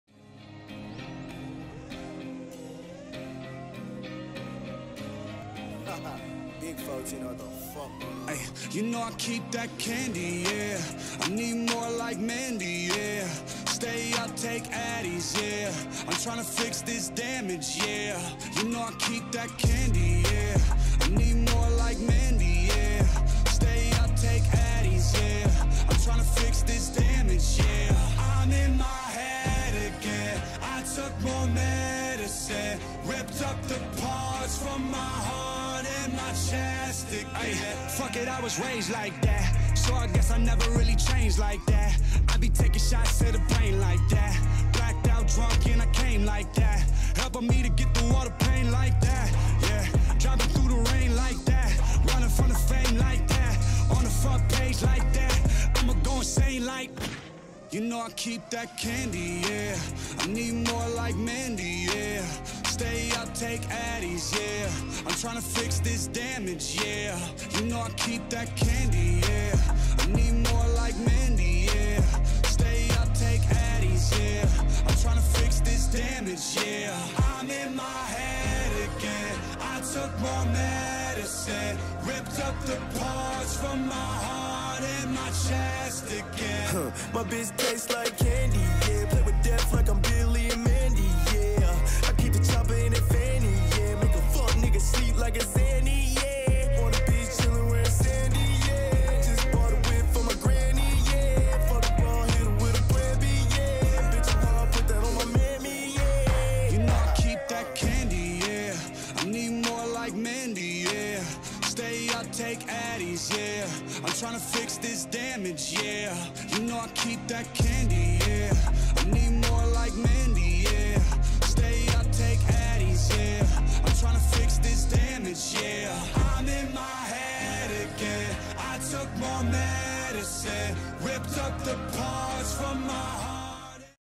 Golden Hour │ MB3Drift │ FPV Freestyle
Little pack fully edited, exported and posted from my iPhone, with iMovie. The LUT is a little extreme given it was already sunset and protuned « Gopro Color » next time I will try flat. But I like that, kinda looks like a knock-off Skitzo^^

Setup Freestyle Red/Green):
- Hyperlite Flowride 5
- Aikon 4in1 35A 6S Blheli32
- 3300uF 30V capacitor
- Hyperlite F4 OSD FC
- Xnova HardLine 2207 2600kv
- Runcam Micro Swift 2 25/30 degres of tilt
- TBS Unify Pro HV
- Menace RC Pagoda LHCP
- TBS Crossfire Micro Rx v2 horizontal mount
- HQprops 5.1x3.1x3 V1S/ Ethix S3
- CNHL 1330 4S 100C (white)
- Brain3D Session 5 mount 30 degres of tilt, arm guards, and pigtail mount (reversed)